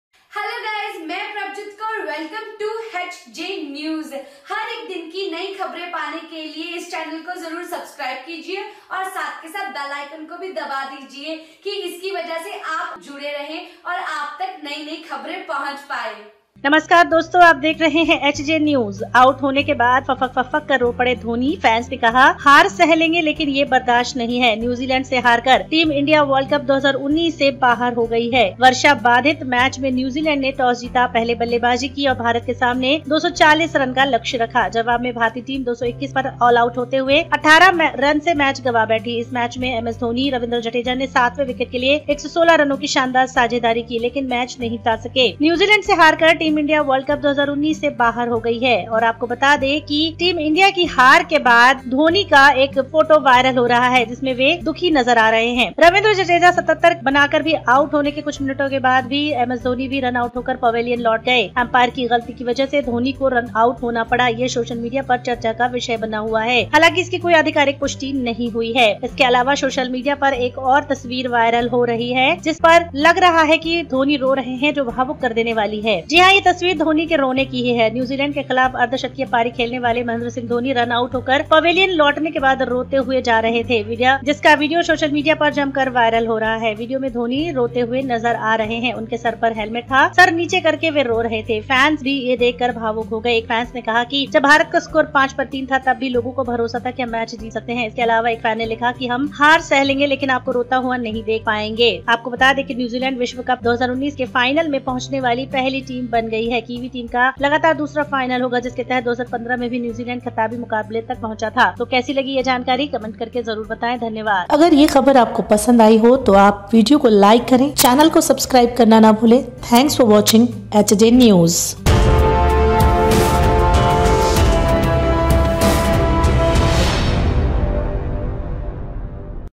आउट होने के बाद फफककर रो पड़े धोनी | Ms Dhoni Crying | HJ News
HJ NEWS !!